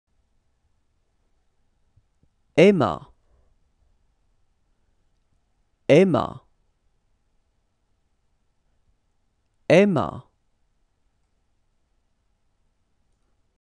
Prénom de filles en français # EMMA
Apprenez à prononcer et à écrire de nombreux mots avec Vincent. C'est simple: lisez, écoutez et répétez les mots que vous entendez !